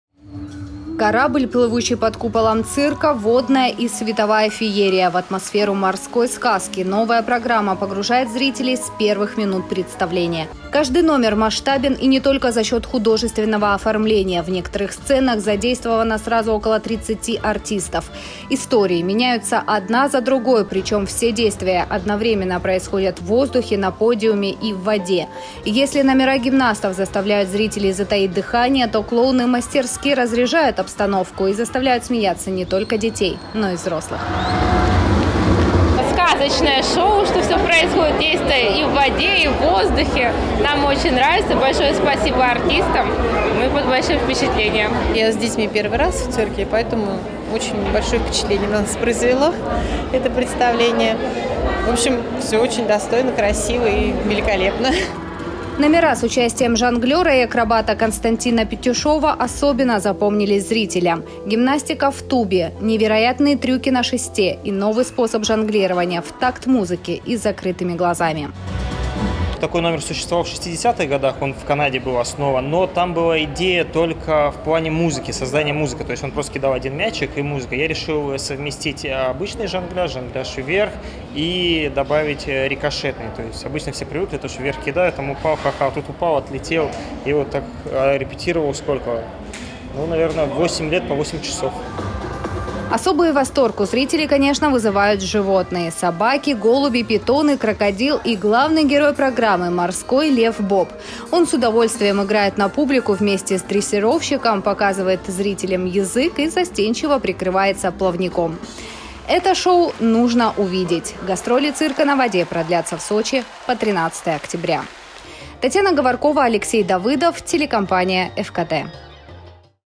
В Сочи состоялась премьера цирка на воде «Шевченко-шоу». Новости Эфкате
В минувшие выходные состоялась премьера новой программы цирка на воде. Она прошла при полном аншлаге.

Корабль, плывущий под куполом цирка, водная и световая феерия: в атмосферу морской сказки новая программа погружает зрителей с первых минут представления. Арену цирка превратили в бассейн.

Каждый номер масштабен не только за счет художественного оформления. В некоторых сценах задействовано сразу три десятка артистов. Истории меняются одна за другой. Действие одновременно разворачивается в воздухе, на подиуме и в воде. И если номера гимнастов заставляют зрителей затаить дыхание, то клоуны мастерски разряжают обстановку и заставляют смеяться не только детей, но и взрослых. Номера с участием жонглера и акробата Константина Петюшова особенно запомнились зрителям. Гимнастика в тубе, невероятные трюки на шесте и новый способ жонглирования — в такт музыке с закрытыми глазами.